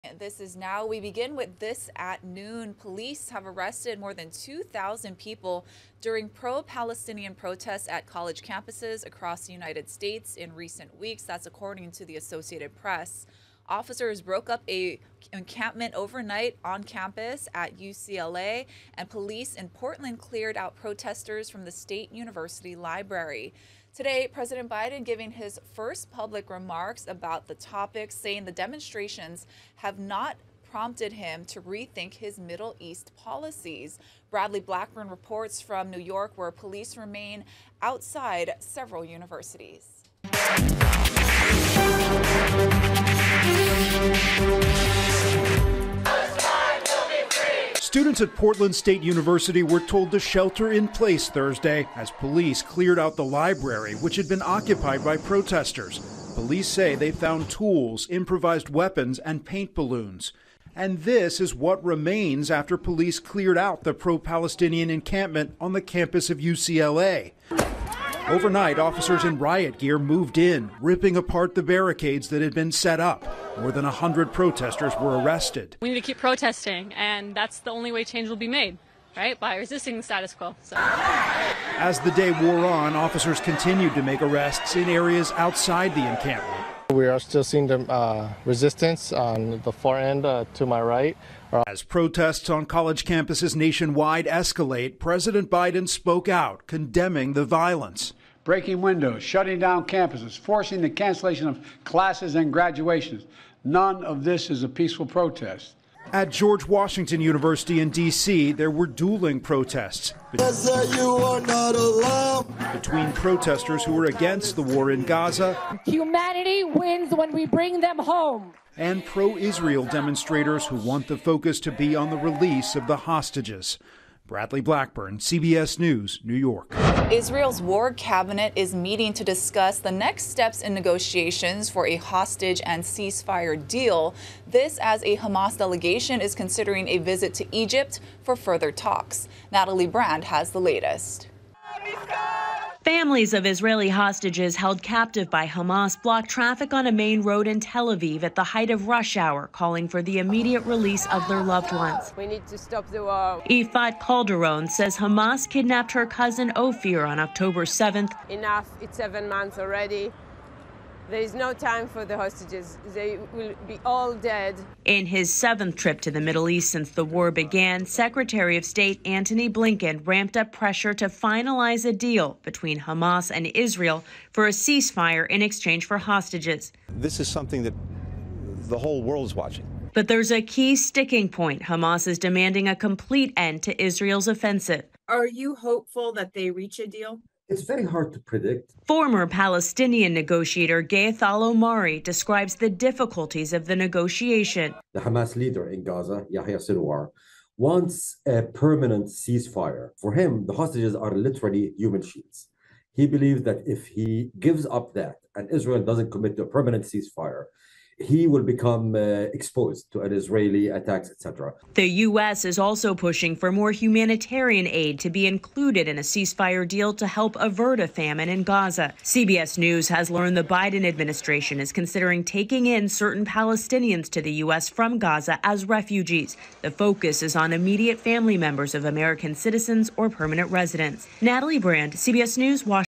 At least 2,000 people arrested at pro-Palestinian protests on US campuses, AP tally shows
Police have arrested more than 2,000 people during pro-Palestinian protests at college campuses across the United States in recent weeks.